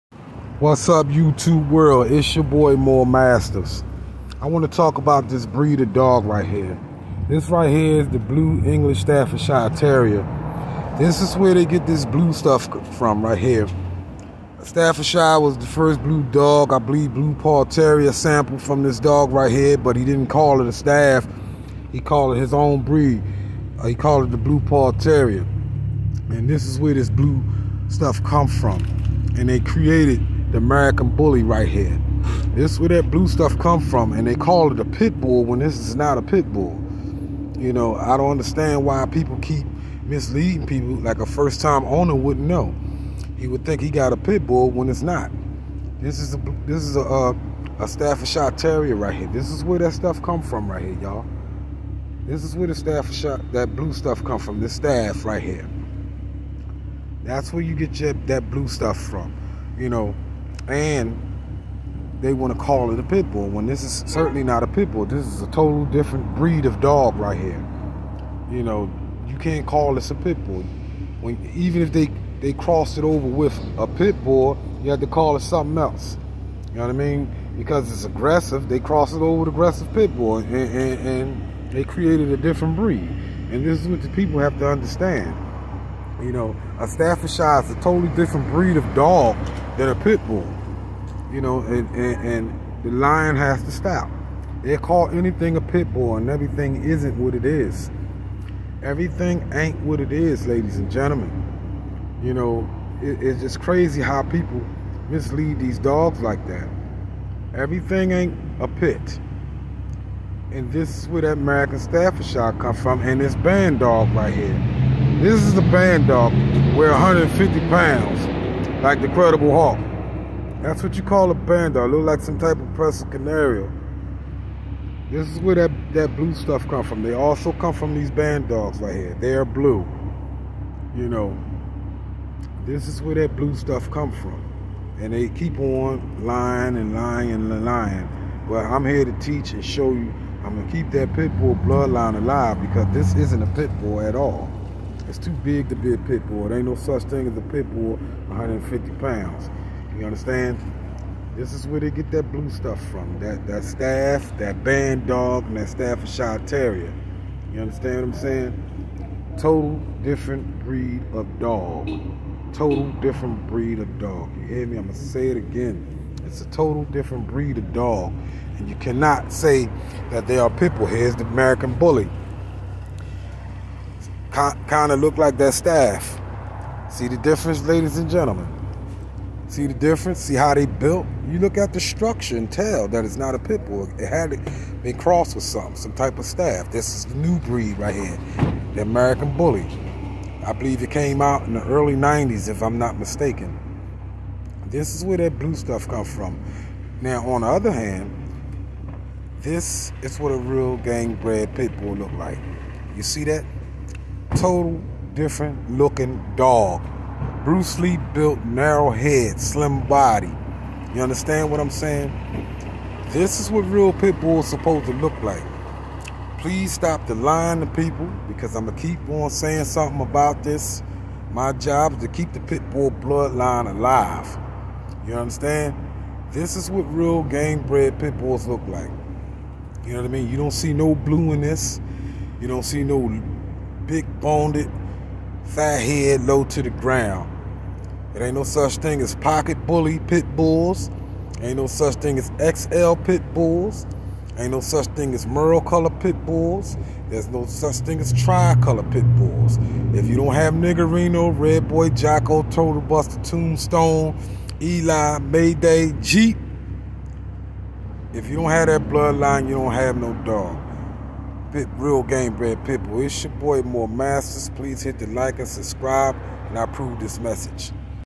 staffordshire terriers is where they got the blue dogs from
hello this is moore mastiffs I have American bulldog cross it over with American bully created my own breed I called my breed American bully bulldogs I don't lie to people mislead dogs I tell the truth not a shame of what I created no different than what they've been doing before I was born if anyone interested in purchasing the puppy you can go to my Facebook page it's called Chuck D